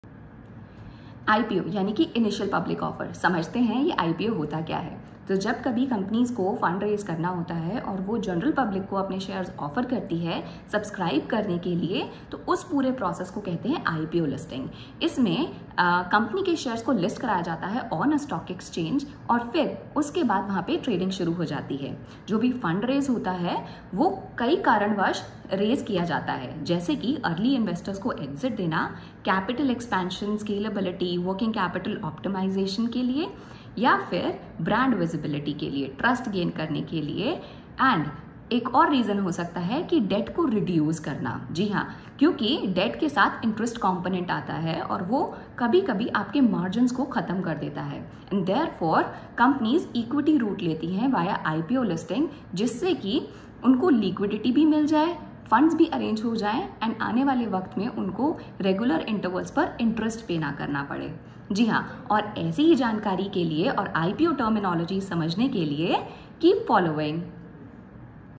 What is an IPO - IPO TERMINOLOGIES #1
Ever wonder what an IPO is and why it matters? 🤔 An Initial Public Offering (IPO) is when a company goes public, offering its shares to investors for the first time. 🚀 It’s a key moment for companies and a big opportunity for investors!

👉 Learn more about how IPOs work and why they’re important for your investment journey! Want to dive deeper into IPO terms and strategies? Follow us for more easy-to-understand financial insights! 🔍💼